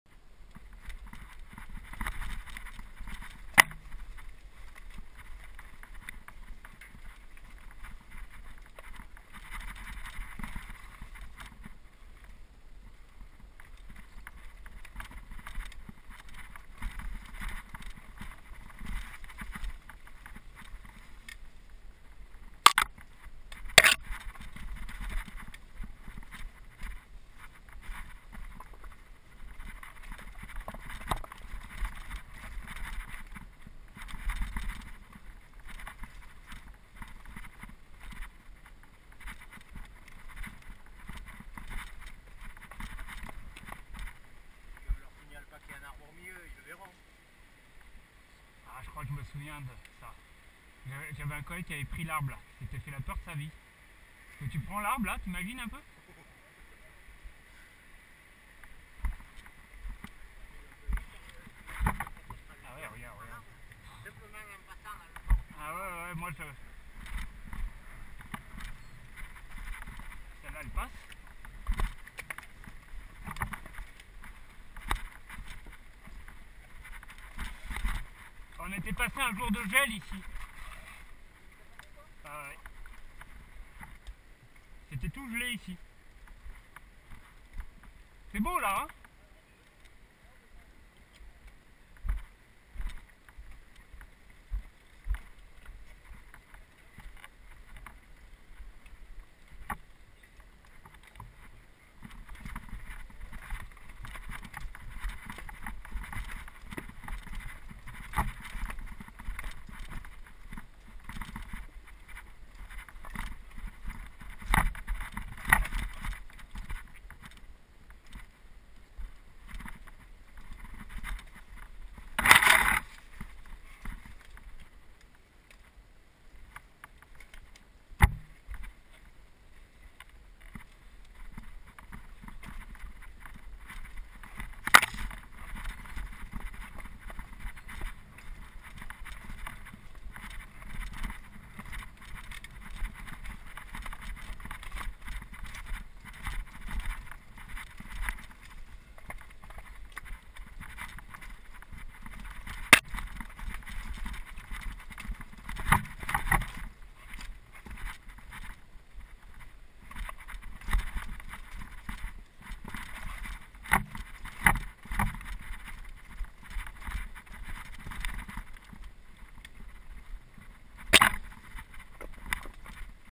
VTT sur les pentes de Malons et Elze entre Gard, Ardèche et Lozère - 09
- Reconnaissance et balisage du parcours de Malons et Elze (30) pour la Randonnée VTT du 11 aoôut 2013